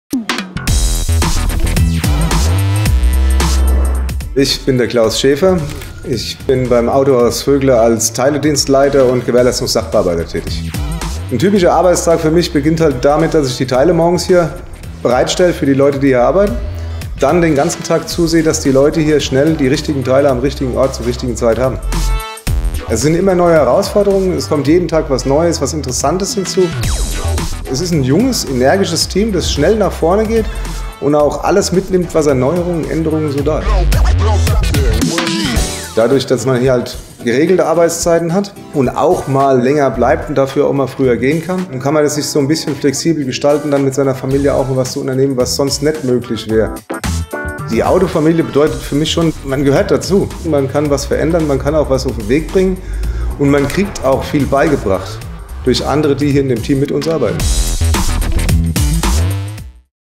Unsere Autohaus-Helden: Klaus Schäfer Teiledienstleiter
Ein weiterer Autohaus-Held des Autohaus Vögler stellt sich vor. Unser Teiledienstleiter Klaus Schäfer.